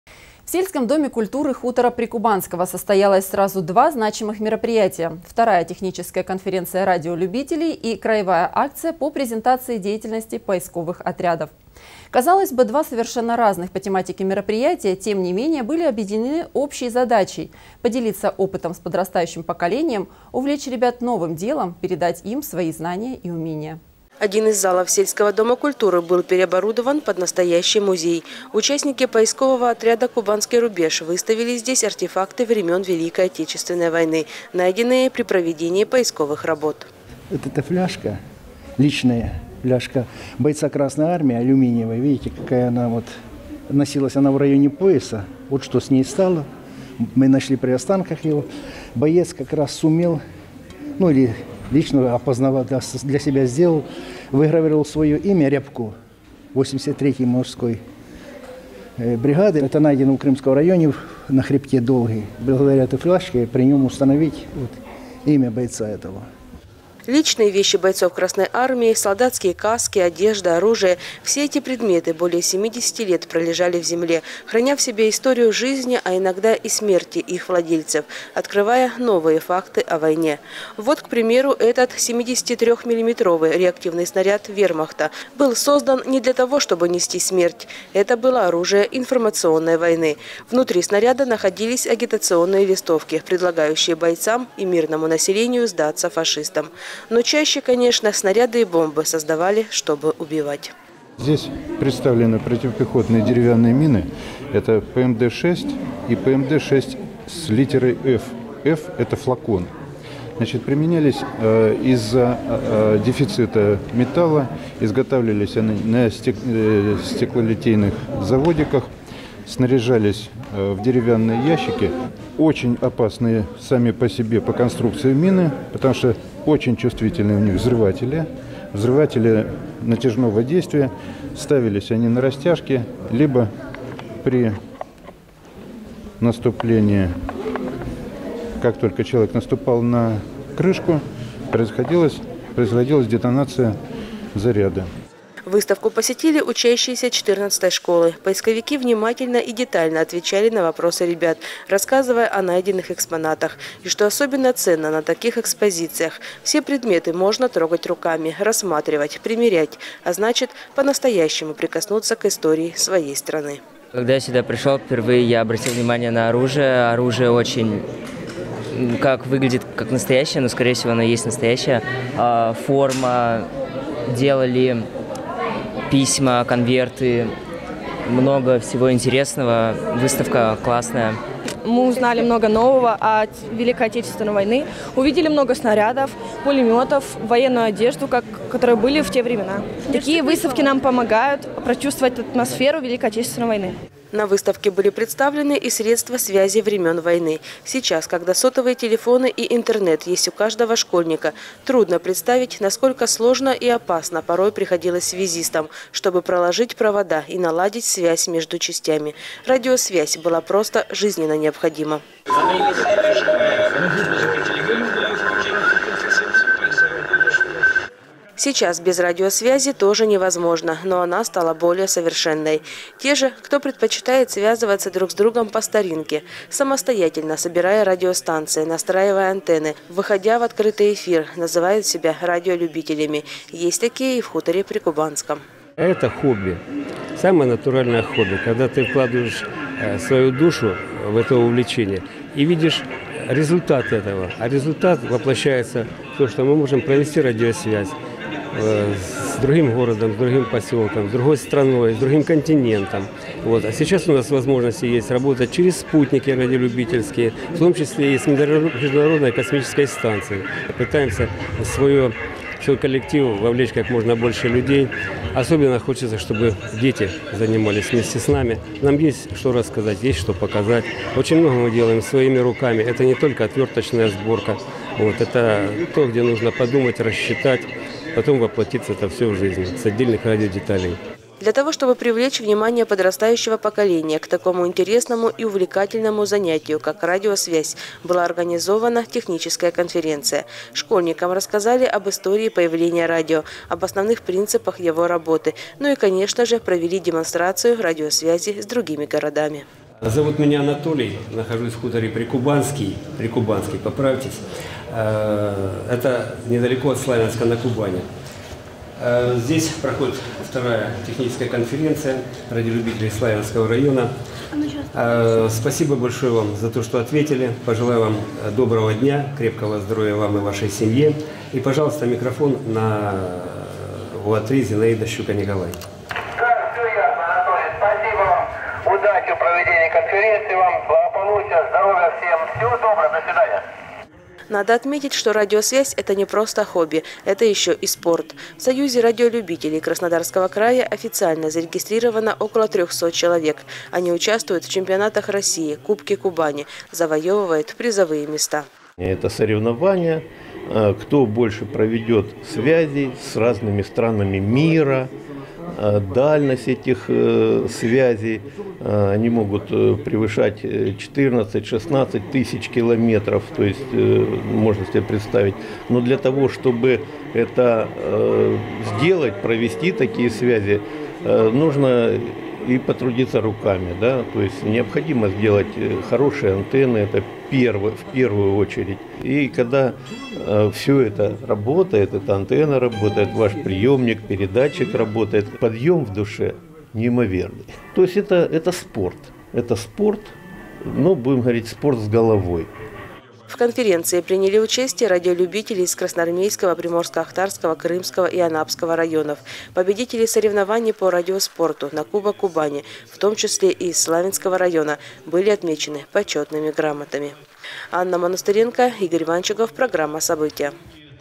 На одной волне
В сельском доме культуры хутора Прикубанского состоялось сразу два значимых мероприятия- вторая техническая конференция радиолюбителей и краевая акция по презентации деятельности поисковых отрядов.  Казалось бы, два совершенно разных по тематике мероприятия, тем не менее, были объединены общей задачей- поделиться опытом с подрастающим поколением, увлечь ребят новым делом, передать им свои знания и умения.